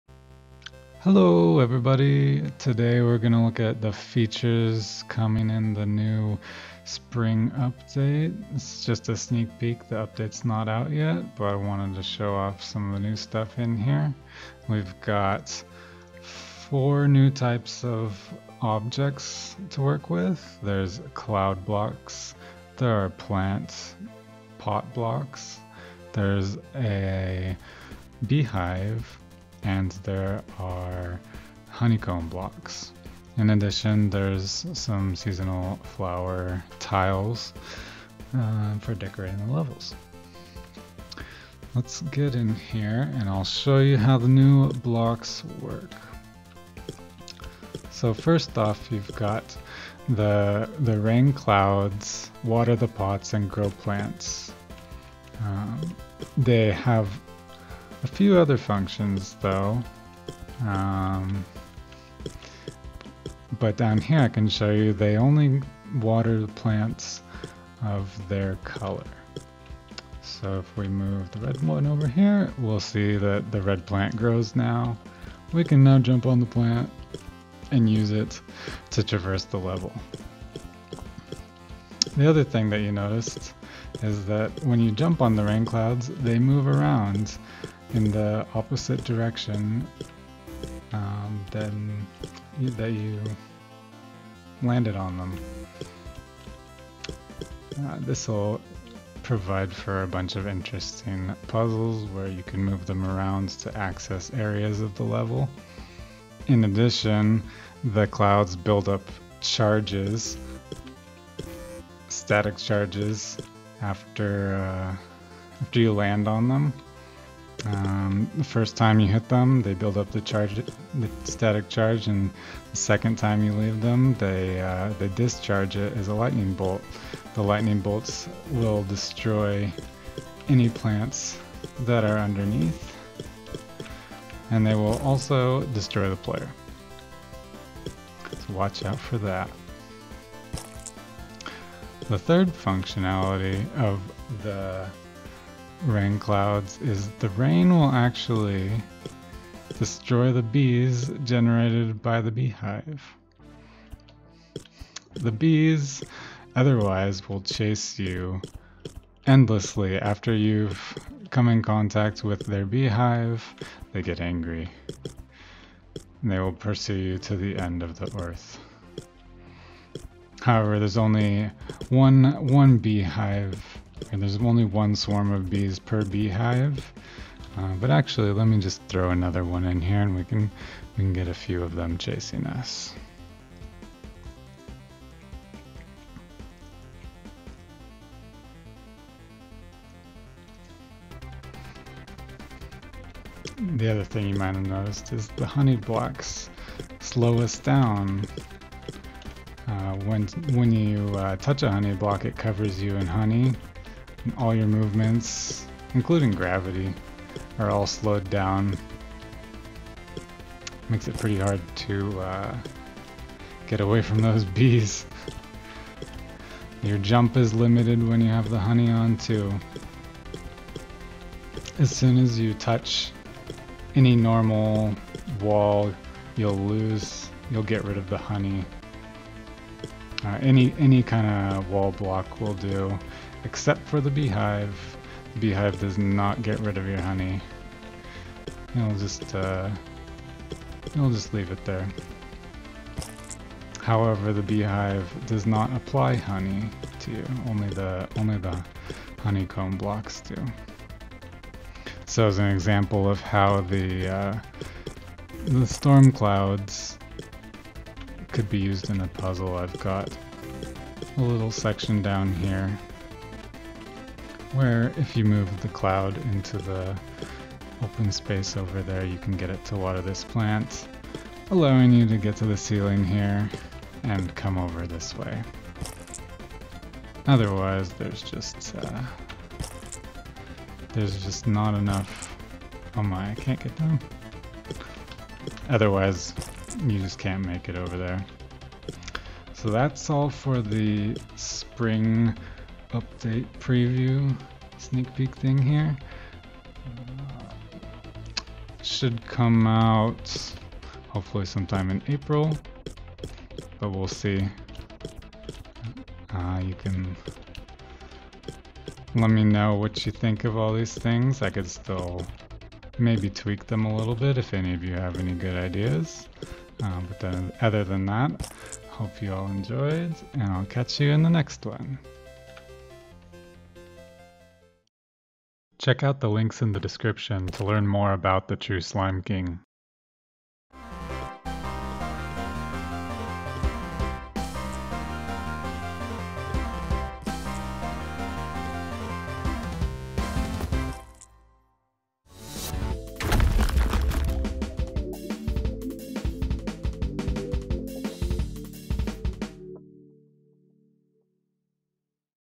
Spring Update Sneak Peek | Devlog #5
In this video, we go over the functions of the new objects in the upcoming spring update. Spring is the only season left without themed puzzle elements, so with this update the game will be well rounded. This update will bring four new blocks to play with: storm clouds, potted plants, beehives, and honeycomb.